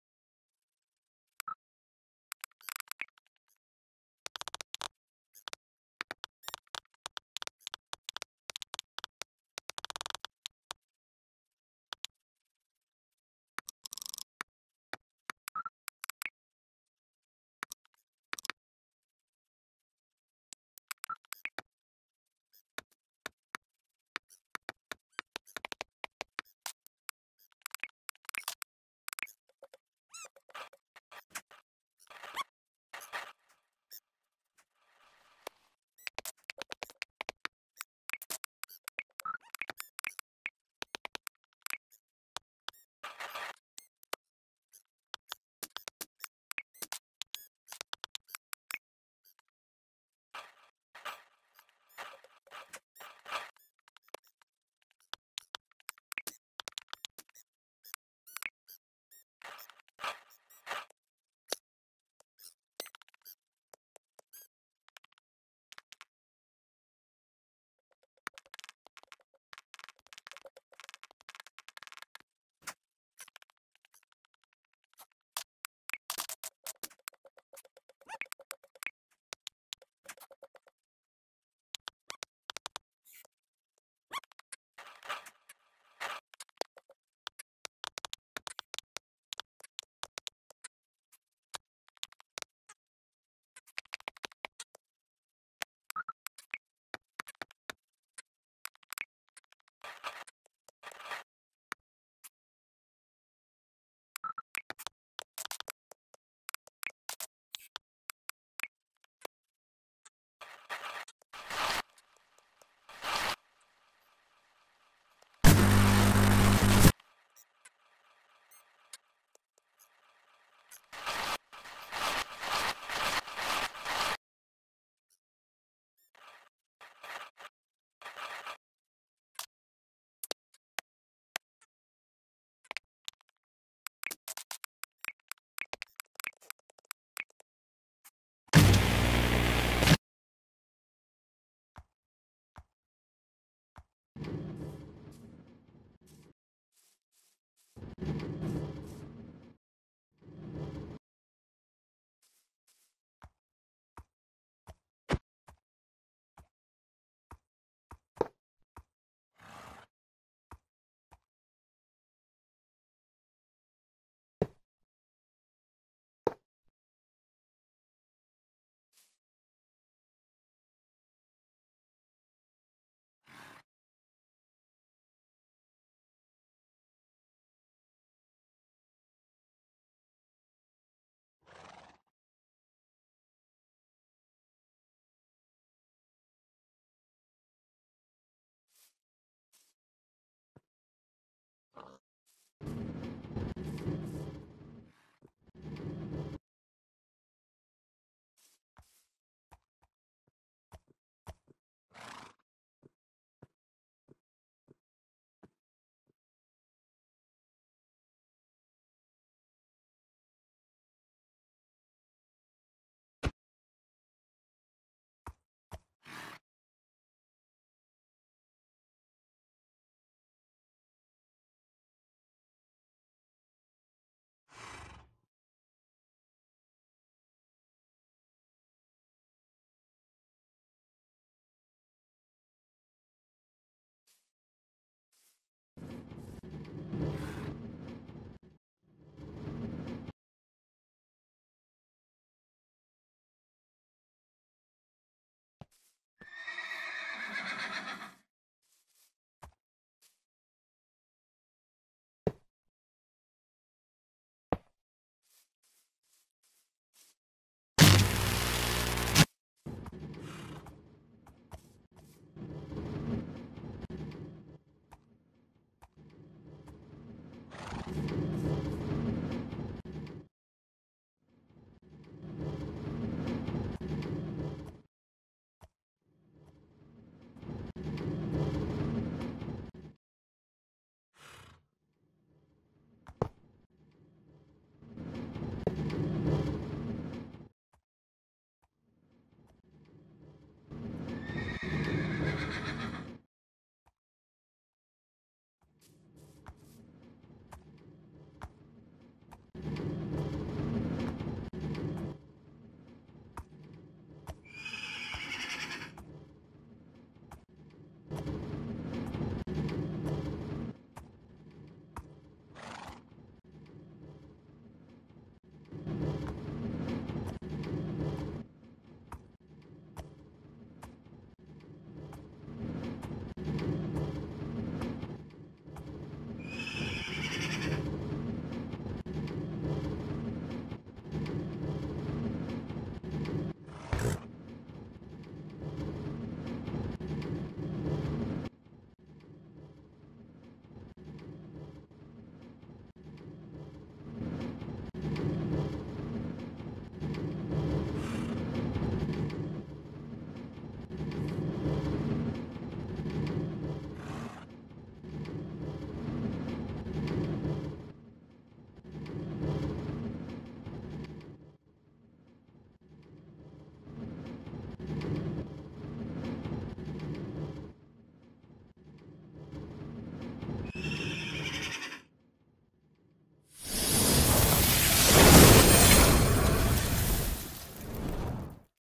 Horse Riding Cart
This is a video of me making a rail system for a town or city that I'm creating in my free time and while doing it two horses decide to take a joyride around the rail system that I was testing